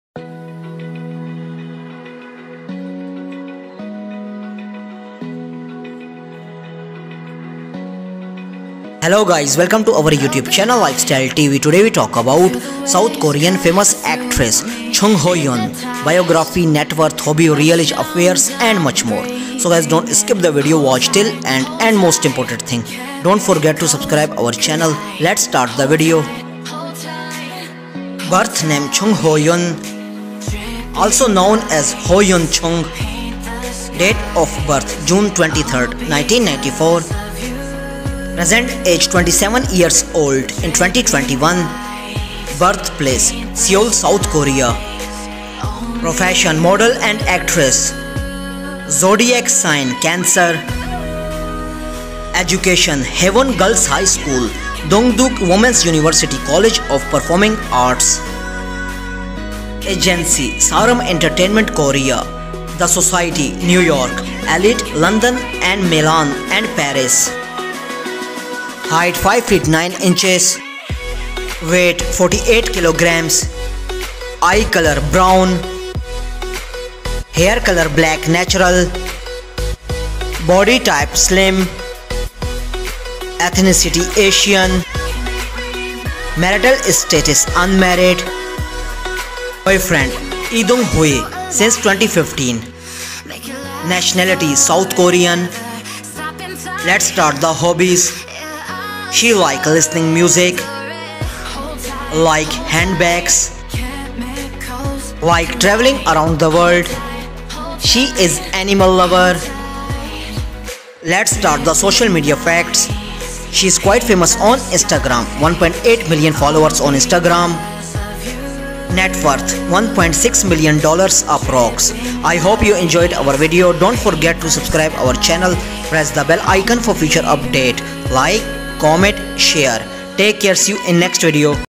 Jung Ho Yeon Lifestyle (Squid Game) Real Life Partner, Biography, Age, Net Worth, Hobbies, Facts
Squid Game Actress Lifestyle (Squid Game 2021) Real Life Partner, Biography, Age, Income, Hobbies, Net Worth, Height, Weight, Facts

HoYeon Jung (born Jung Ho-Yeon) is a South Korean fashion model. Vogue has dubbed her as “Korea’s next top model”, and she appeared on the namesake show’s 4th season (where she was placed second). She currently ranks as one of the “Top 50” fashion models by models . com.